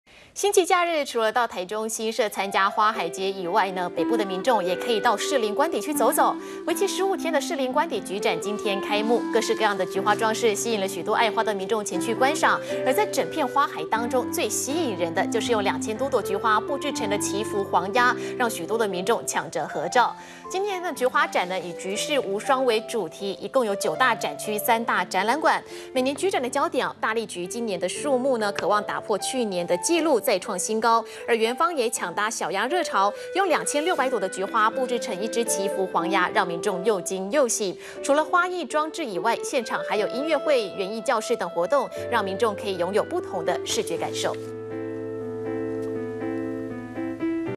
【2013.11.17】士林官邸菊展開幕 祈福黃鴨吸睛 -udn tv
為期15天的士林官邸菊展今天開幕，各式各樣的菊花裝飾吸引許多愛花的民眾前去觀賞。而在整片花海中，最吸引人的就是用2千多朵菊花布置成的祈福黃鴨，讓許多民眾搶著合照。